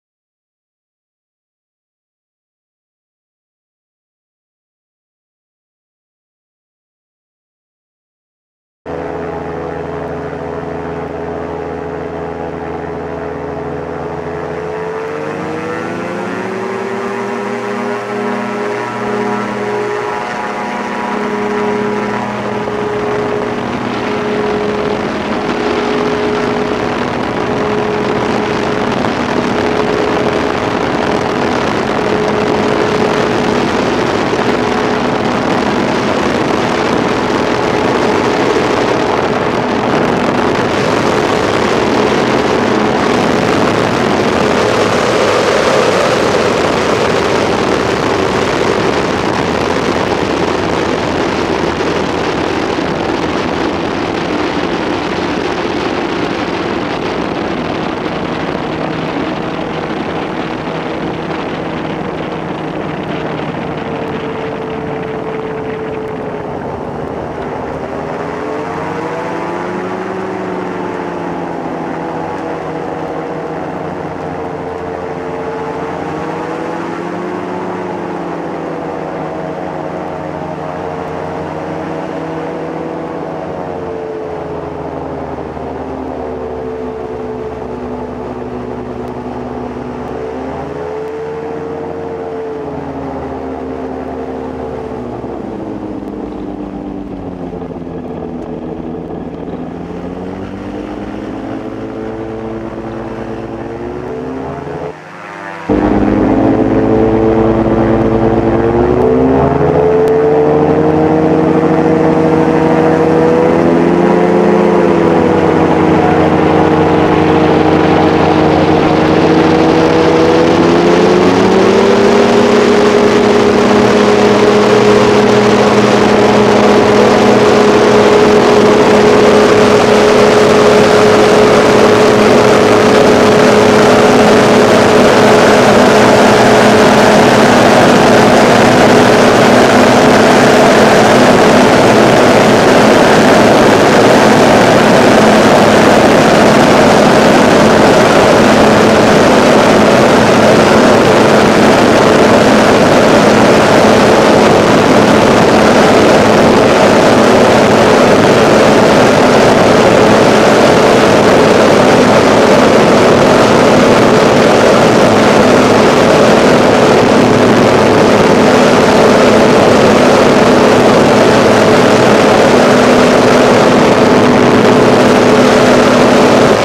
downwind take offs are not for fat guys.
This video is about downwind take off